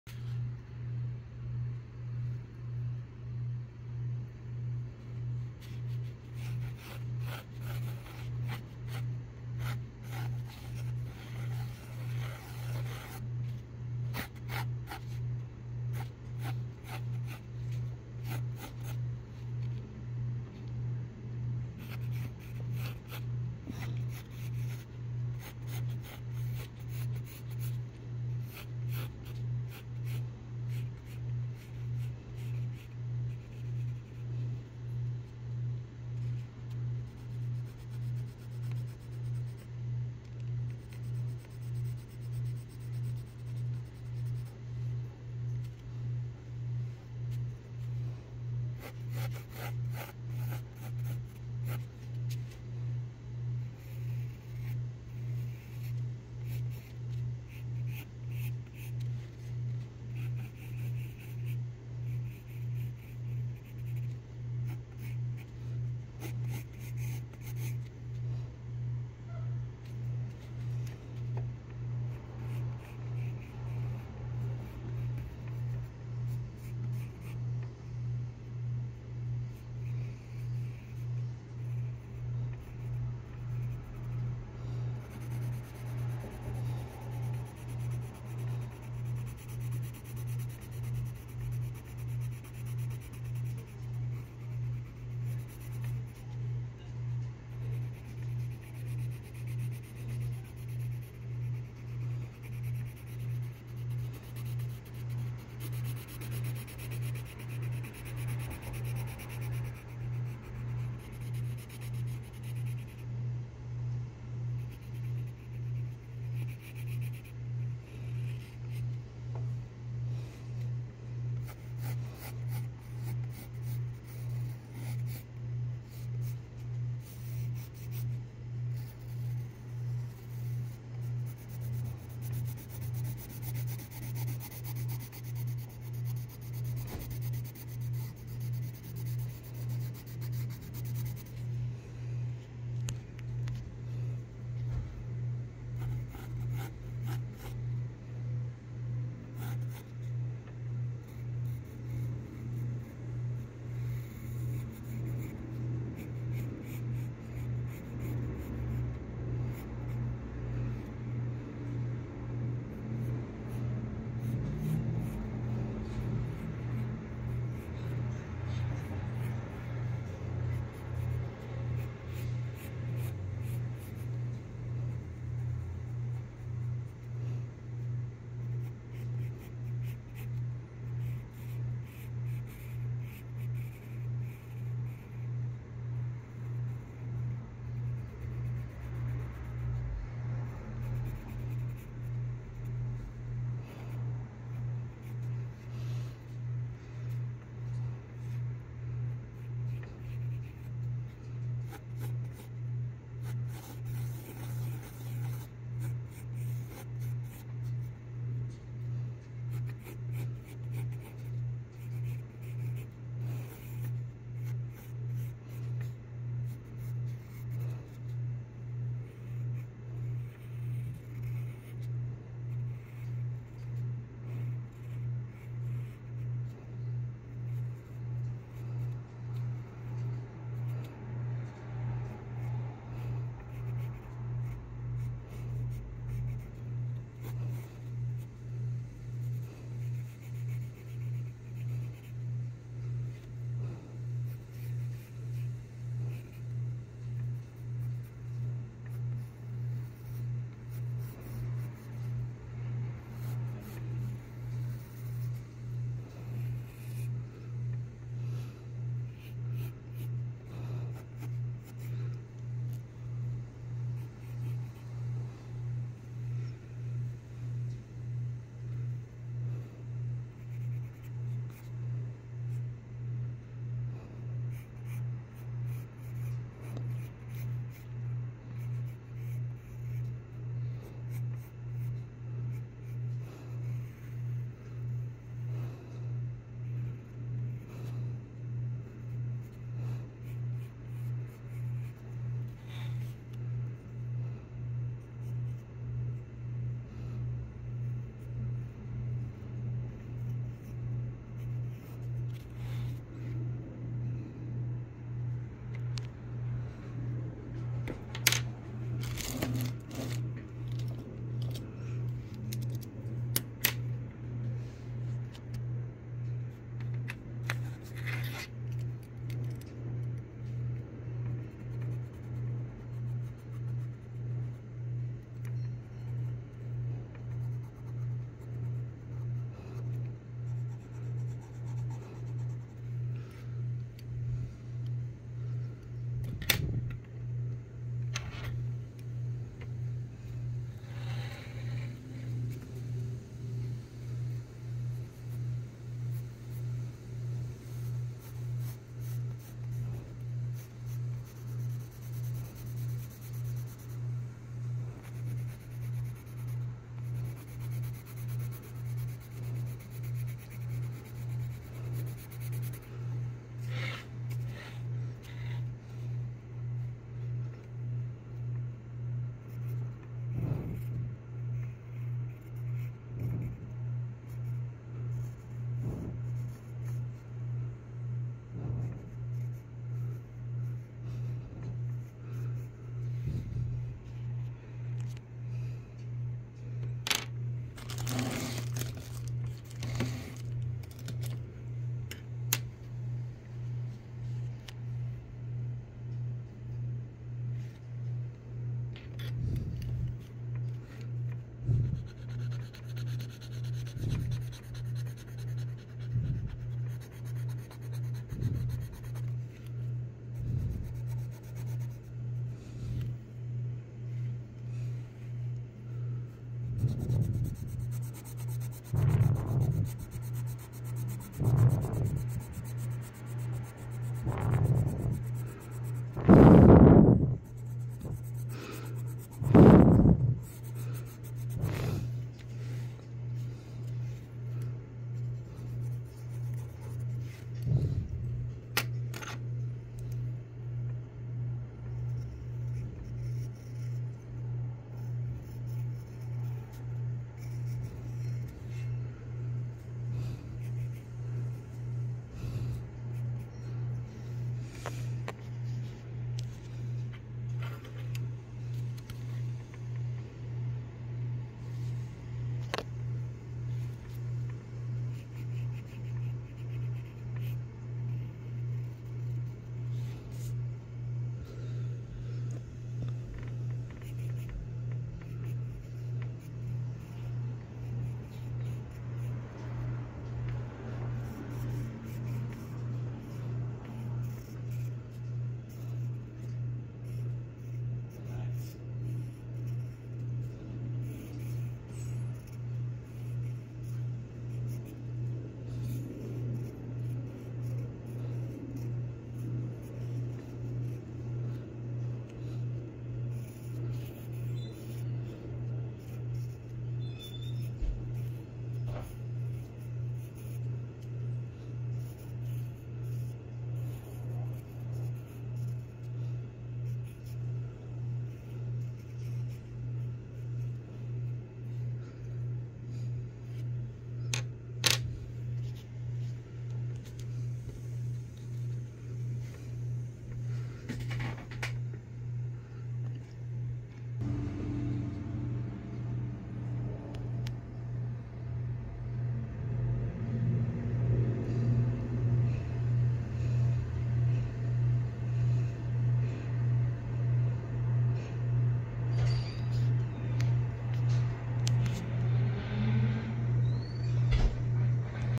@ChikinNuggit- Drawing I Scream 🐰👹
A drawing video of @chiknnuggit 's I Scream. The cute and scary trouble making Bunny Boy.